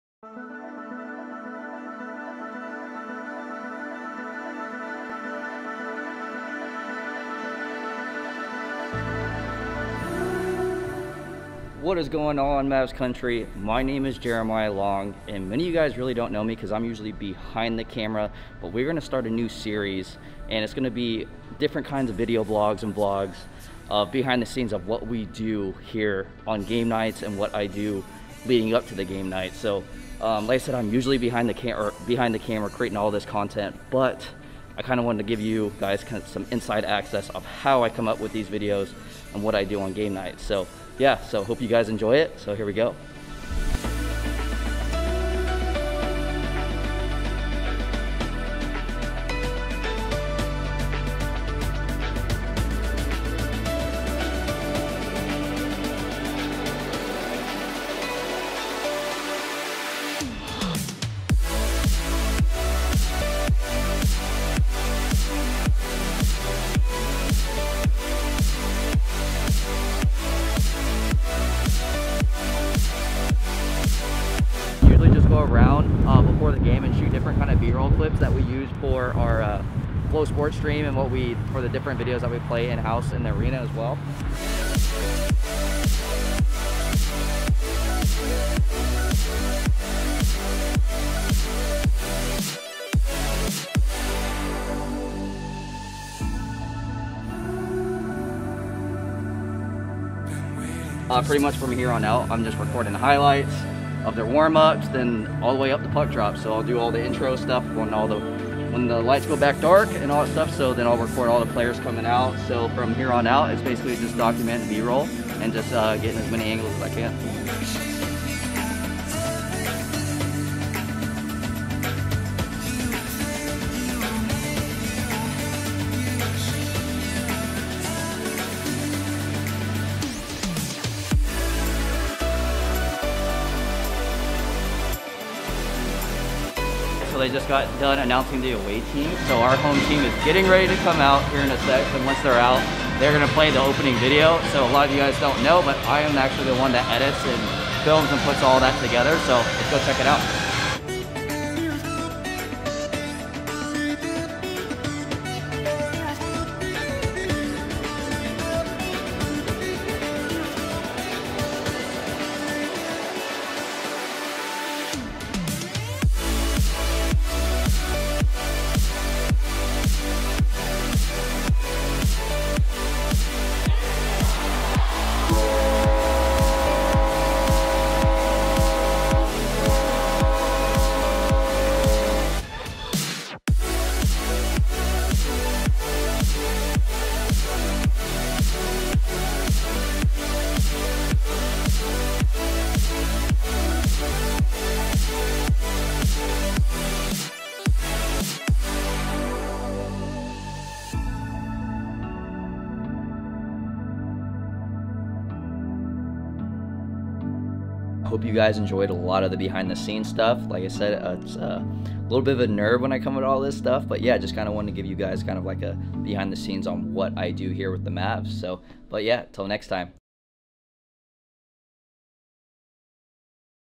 Kansas City Mavericks Video Production l Behind the Scenes
On this first segment of our behind-the-scenes features,  we follow along with the manager of video production and content, Jeremiah Long.  He films, edits, and produces the Kansas City Mavericks video content that features on social media platforms, Flo Sports, and opening video segments.